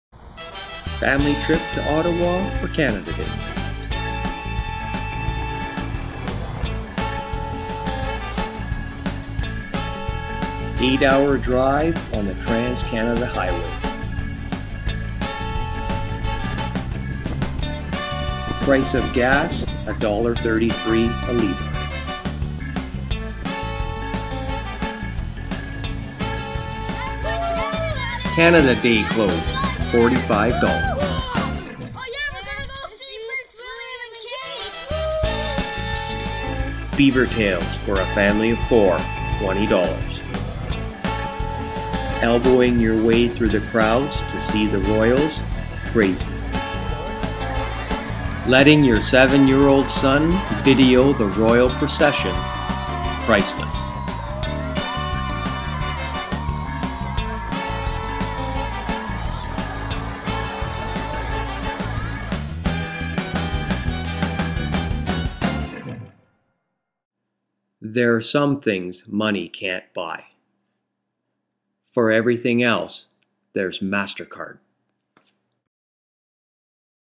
The Royals in Canada for Canada Day... "Priceless"
Family trip to Ottawa to see The Royals on Canada Day!
7 year old son missed the procession!